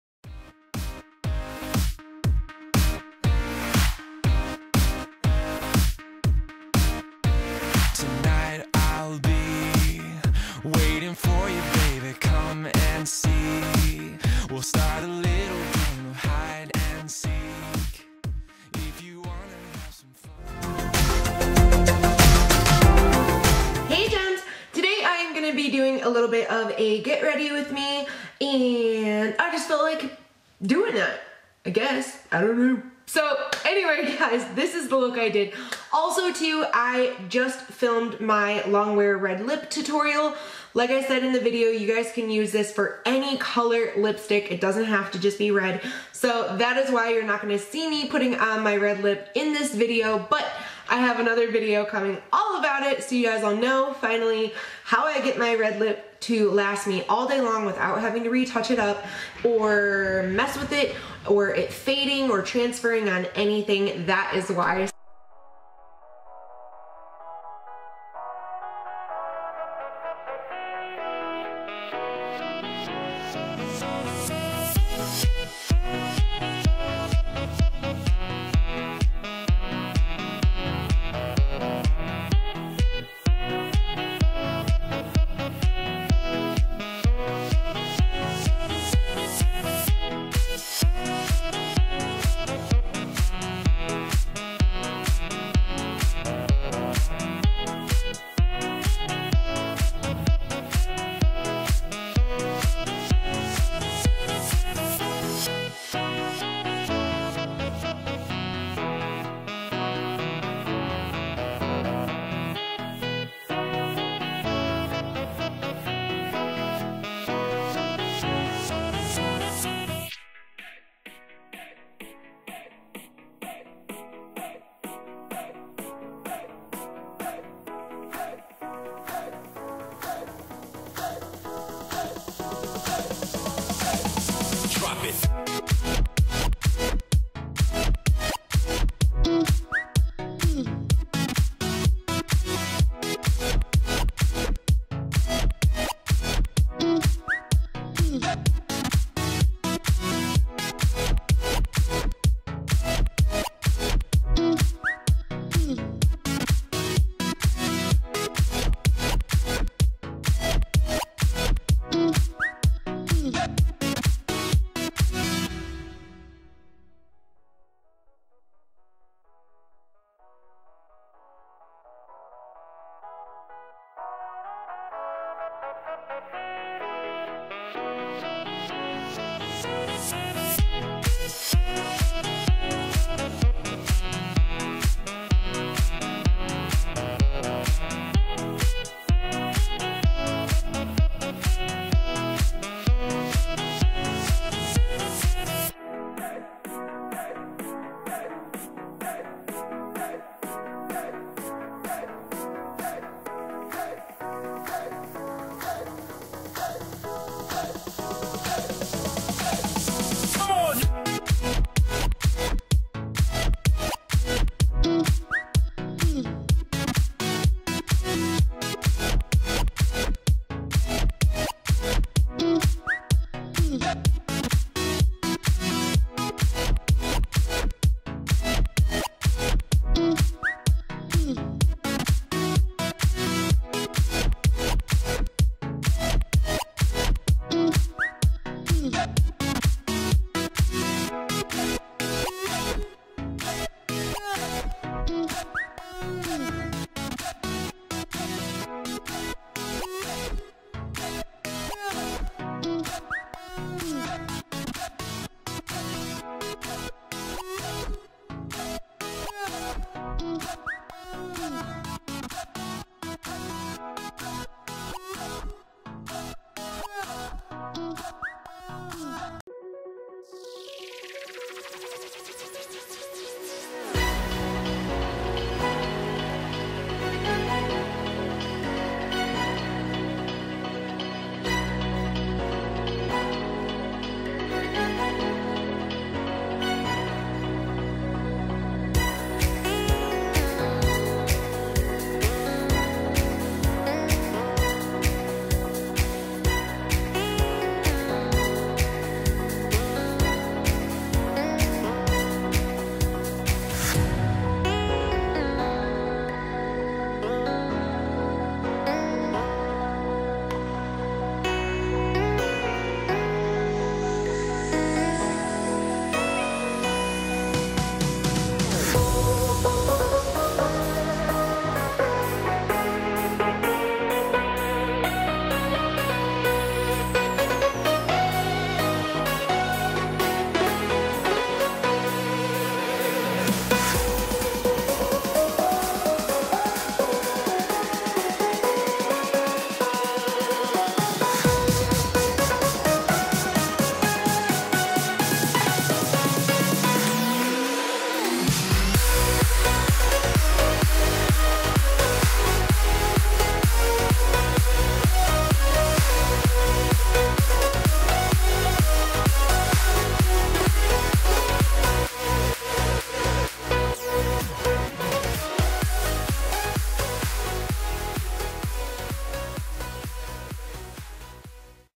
GRWM Makeup for Filming YouTube Videos!
***OPEN ME***

Hello my gorgeous beauties! Today I decided to turn on the camera while I did my makeup to film some videos for you guys and I thought you might enjoy it!

Makeup Used:
~NYX Pore Filler
~L'Oreal Infallible 24 hour Foundation
~MAC Painterly Paint Pot
~Kat Von D Shade & Light Eye Palette
~Sephora Collection Colorful Eyeshadow Sunbeached Filter Palette
~NYC Liquid Eyeliner
~NYX Matte Black eyeshadow
~Urban Decay 24/7 Glide on Pencil in Perversion
~NYX Brow Shaper Wax Pencil
~NYX Eyebrow Cake Powder in Brunette
~Maybelline Brow Precise Perfecting Highlighter
~Maybelline Instant Eye Eraser Concealer in Brightening
~CoverGirl Out Last Concealer
~RCMA No Color Translucent Setting Powder
~Benefit Hoola Bronzer
~Benefit Galifornia Blush
~MAC MSF in Soft and Gentle
~Becca Highlight in Champagne Pop by Jaclyn Hill
~NYX Matte Finish Setting Spray

If you want to see how I got my Red Lips check out my Long Wear Red Lipstick Tutorial Here:

~LINKS TO MORE EM'S GEMS VIDEOS~

XO,
Em

*I AM NOT BEING PAID TO MAKE THIS VIDEO AND ALL OPINIONS ARE MY OWN*